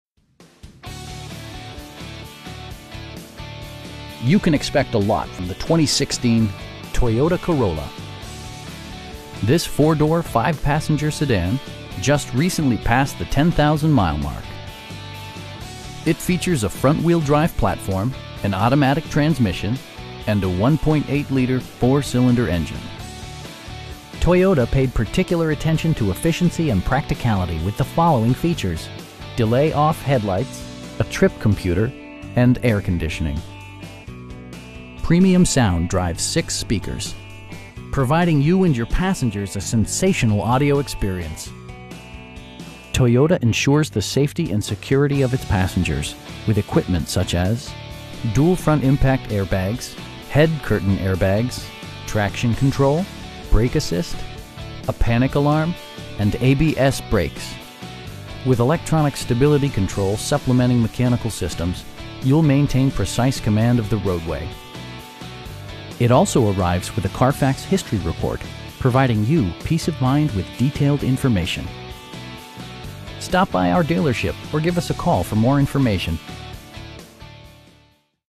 2016 Toyota Corolla LE in Marion, IL 62959
Marion Toyota
3300 W Deyoung

You can expect a lot from the 2016 Toyota Corolla. This 4 door, 5 passenger sedan just recently passed the 10,000 mile mark! It features a front-wheel-drive platform, an automatic transmission, and a 1.8 liter 4 cylinder engine. Toyota paid particular attention to efficiency and practicality with the following features: delay-off headlights, a trip computer, front bucket seats, tilt and telescoping steering wheel, power windows, cruise control, and air conditioning. Premium sound drives 6 speakers, providing you and your passengers a sensational audio experience. Toyota ensures the safety and security of its passengers with equipment such as: dual front impact airbags, head curtain airbags, traction control, brake assist, a panic alarm, and ABS brakes. With electronic stability control supplementing mechanical systems, you'll maintain precise command of the roadway. It also arrives with a Carfax history report, providing you peace of mind with detailed information. We have the vehicle you've been searching for at a price you can afford. Stop by our dealership or give us a call for more information.